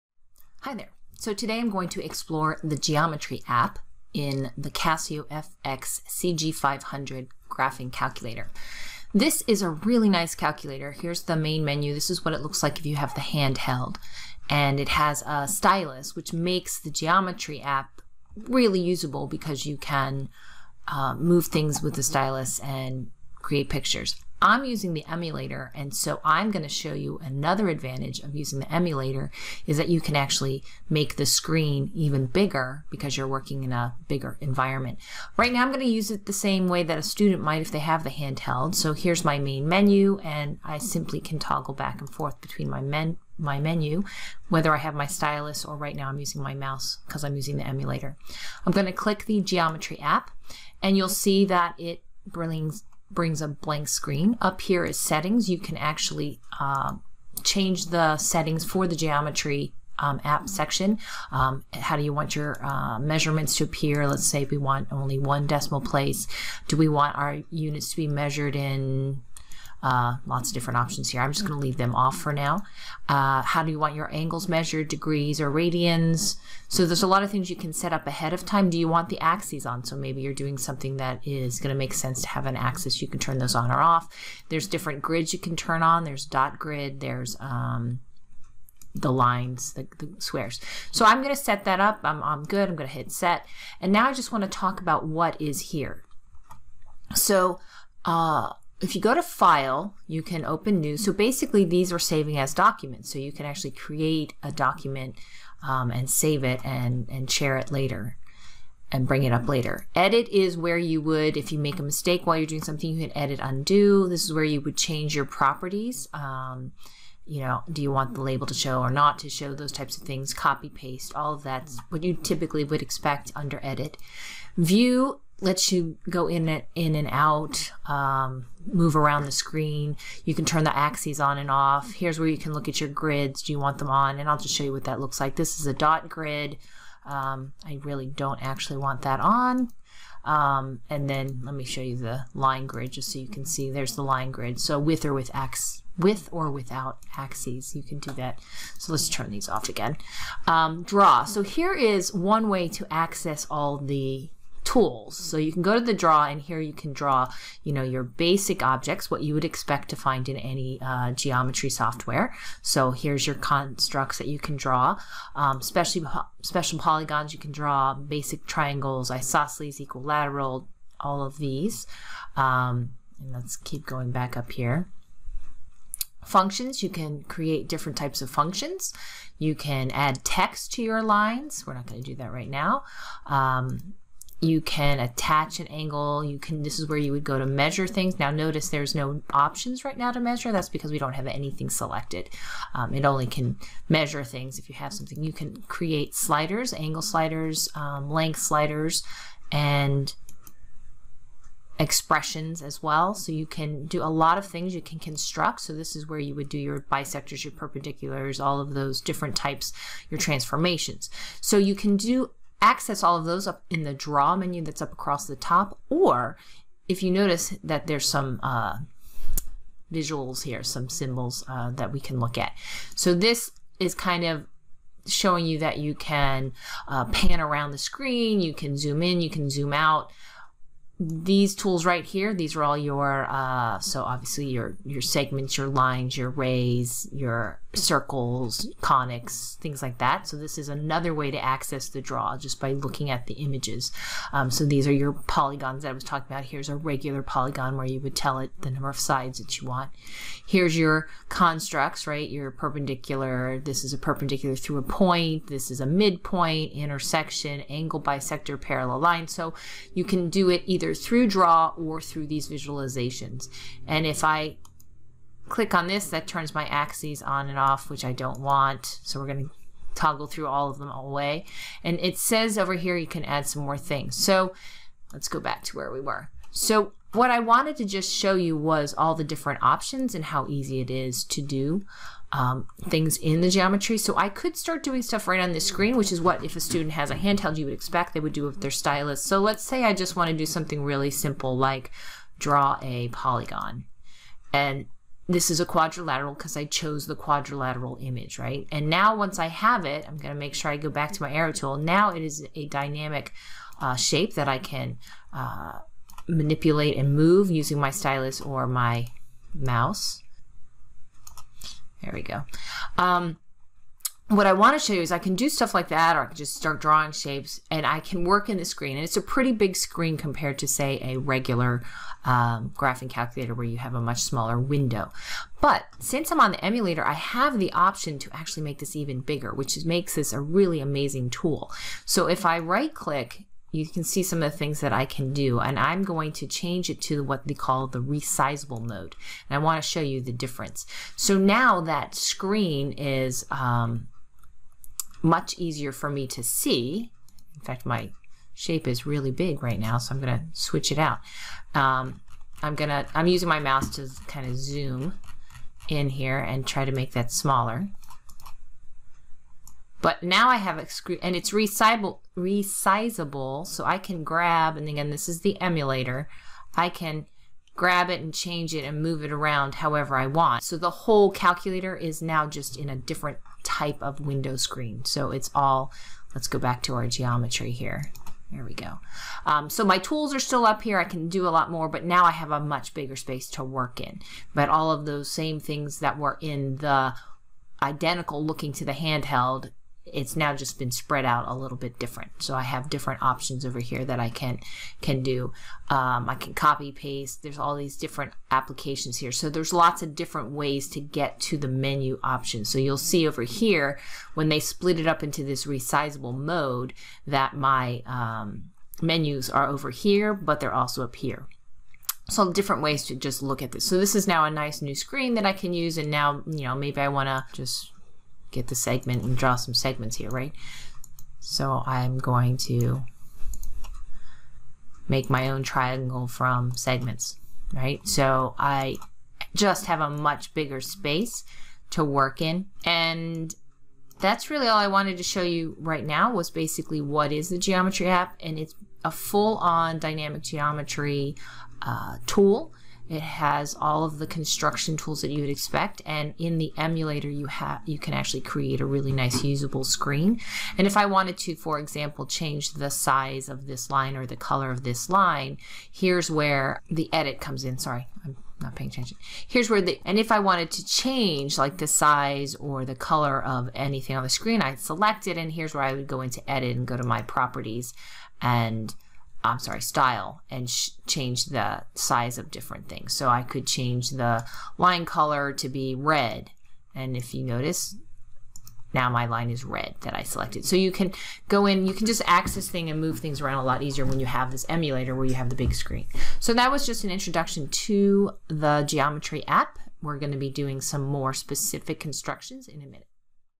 fx CG500: Accessing the Geometry App and What You Can Do
This is an overview of the fx-CG500 Geometry App, which is a full dynamic math tool that allows you to construct geometric shapes, measure angles, lengths - a full geometry software resource. The fx-CG500 is a handheld, but this video also highlights the capabilities of using the emulator software, where you can enlarge the viewing screen, which is great for really exploring geometric constructs.